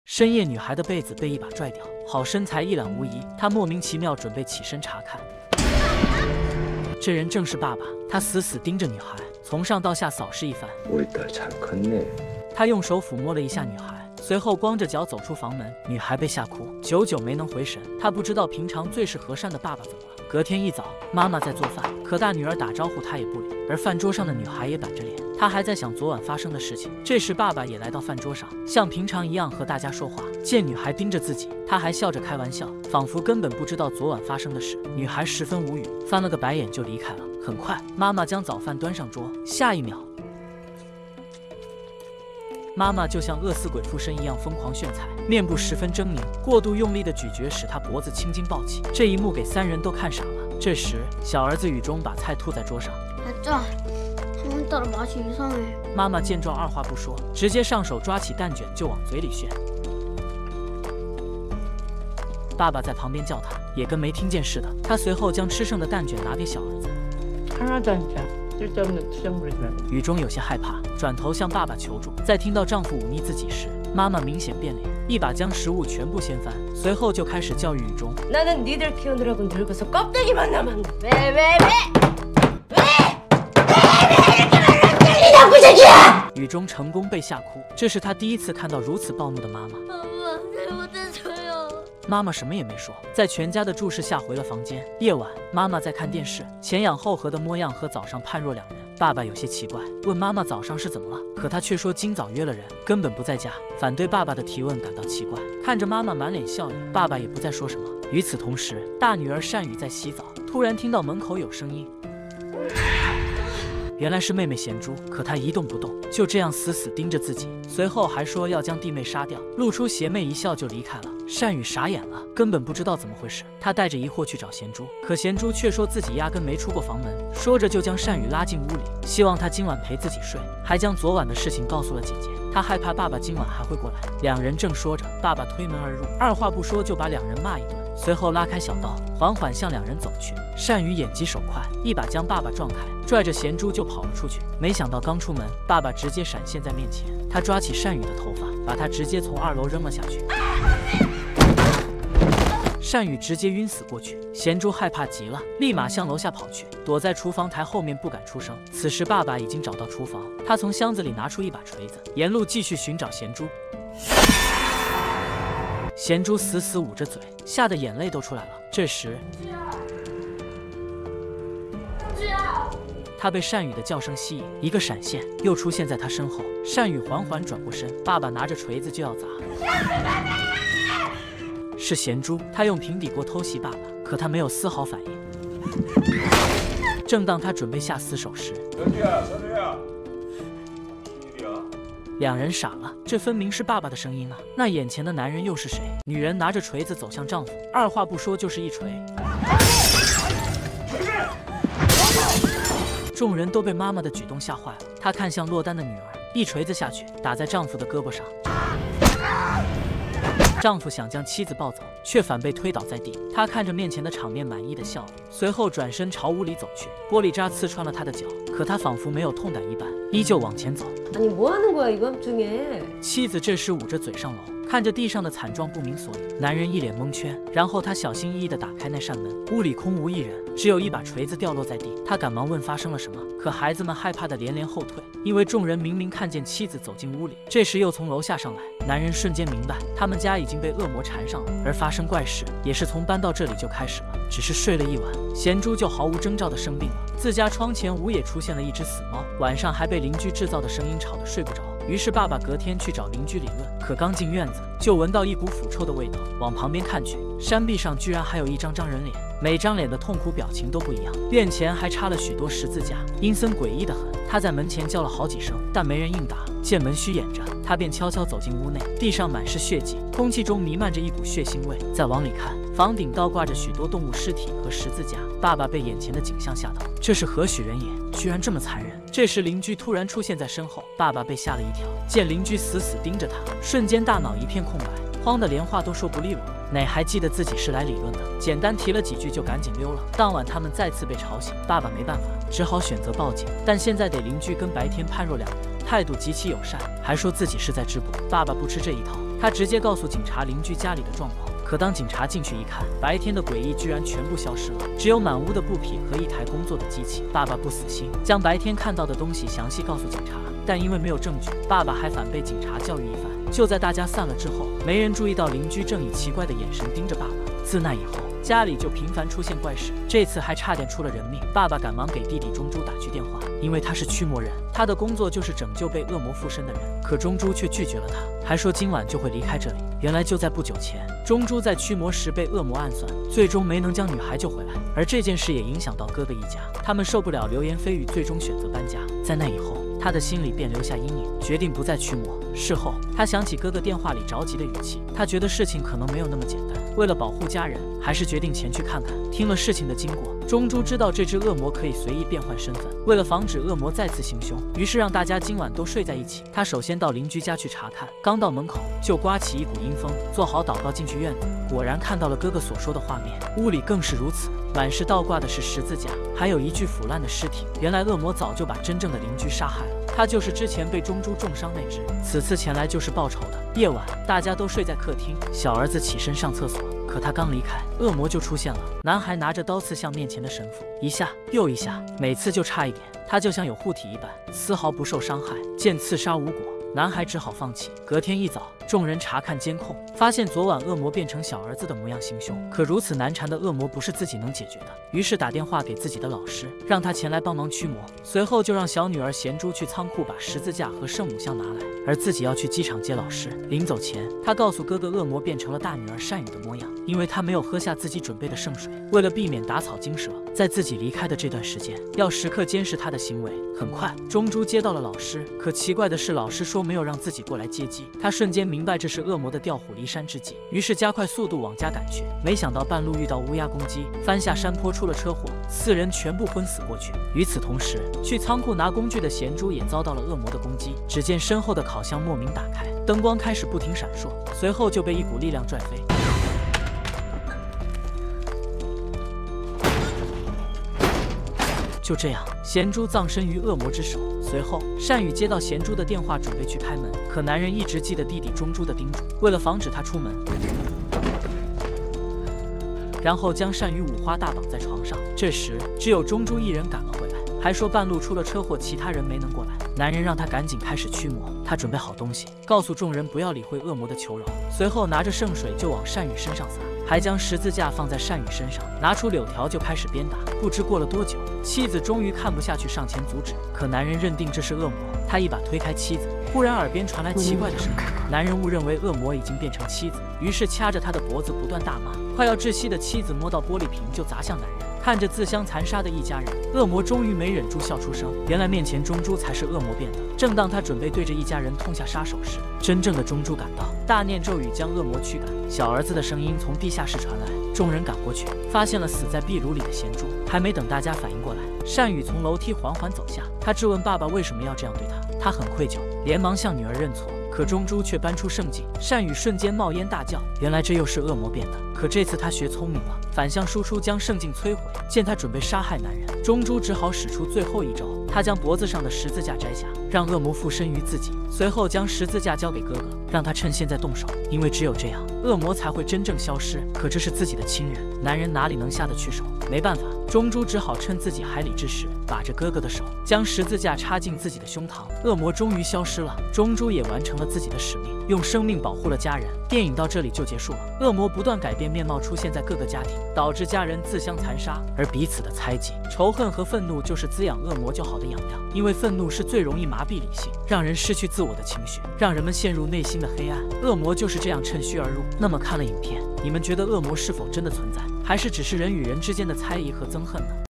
惡魔纏上一家五口，輪流變身下狠手，導致家人自相殘殺
本片簡介：
　  講述了一個神父為了打敗折磨家人的惡鬼而展開的故事。